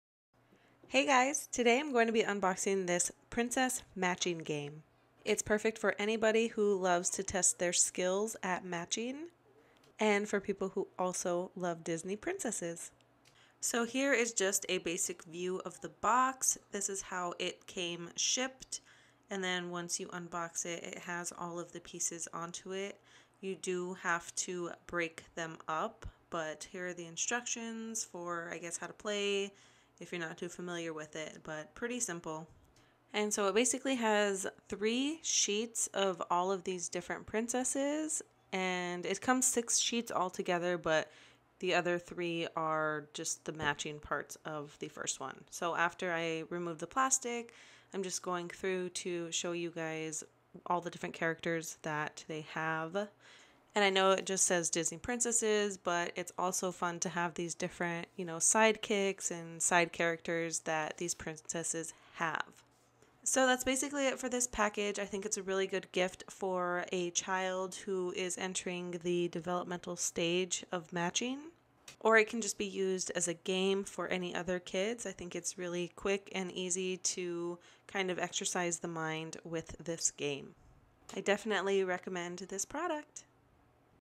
Unboxing Disney Princess Matching Game
A quick video unboxing the Disney Princess Matching Game